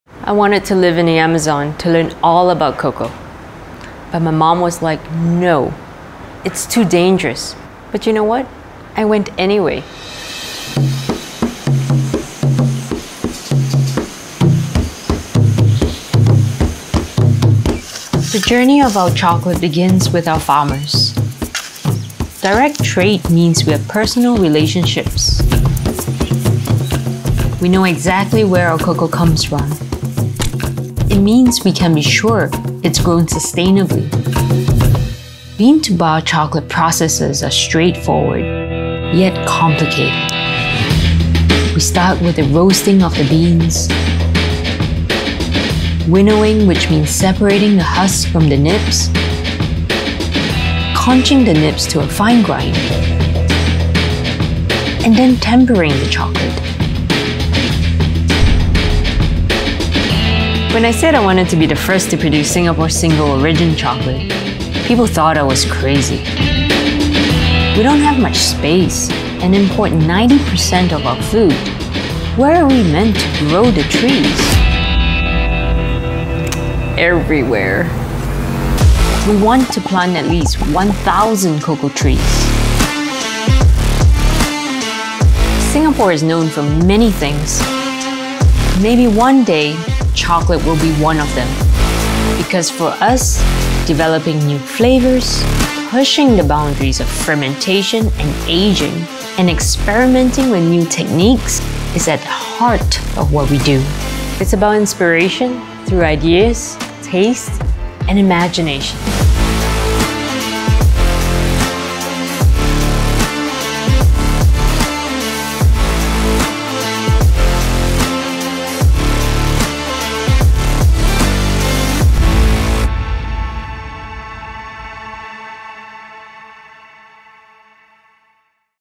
Pure Imagination Bean to Bar Chocolate
Pure Imagination is a small batch, bean-to-bar chocolate focused concept by Janice Wong. From a humble cacao pod, we invite you to tap into what our name suggests, as we explore every step of its journey to become the glorious bar you are about to enjoy today.

We work closely with farmers who’ve become friends in cocoa-rich regions of the Philippines, Ecuador, Thailand, Colombia and Peru. Our ambitions in growing Singapore’s first bean-to-bar chocolate and developing new flavour frontiers through pushing the
boundaries of fermentation and ageing seek to inspire your imagination.

Let us begin this untamed journey into
Pure Imagination.

For the first time ever, Singapore will be home to a true tree to bar chocolate experience — an initiative spearheaded by award-winning chef Janice Wong and her team, who aim to plant 1,000 trees across the island. This is particularly exciting for a country that is land-scarce, and currently imports approximately 90% of food; Janice Wong Singapore’s goal is to transform Singapore into a country known for artisanal chocolate.

“When you think about chocolate in Asia, people generally think of Vietnam, the Philippines, and Indonesia. I’ve always been fascinated by the adaptability and creativity of chocolate as a medium and a craft, and it will be a years-long project to eventually fulfill our dream of a true made-in-Singapore, bean-to-bar chocolate,” says Chef Janice.